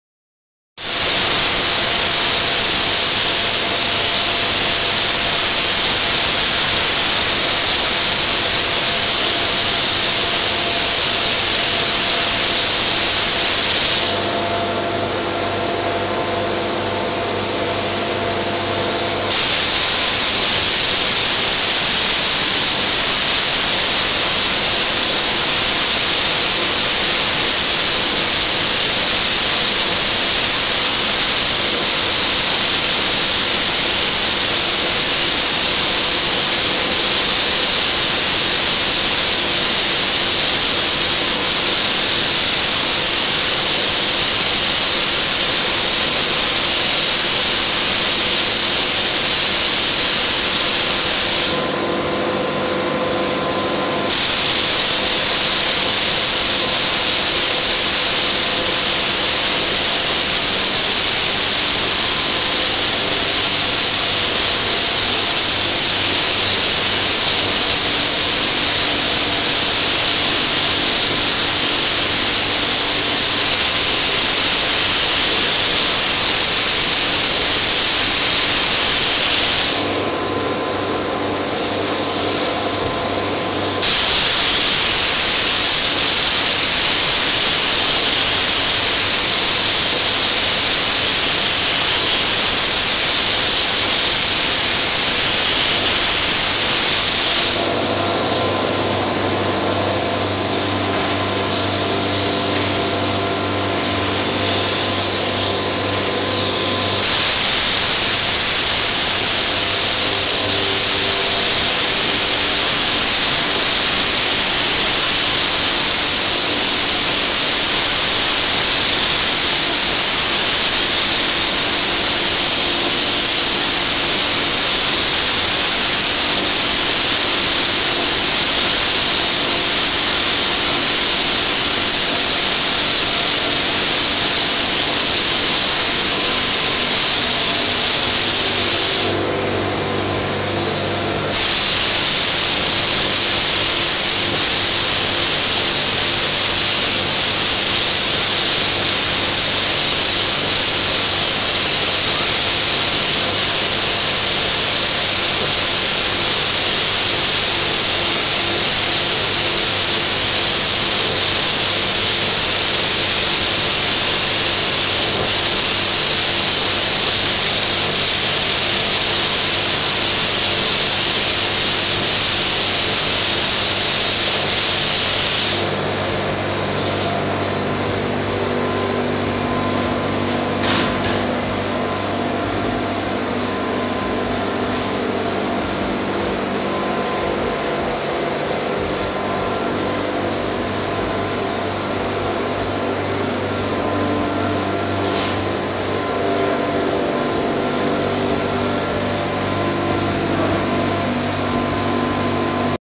Bonnet Spraying 2
After fabrication  a custom bonnet for an Audi S4 it was time to hit it with some paint :-)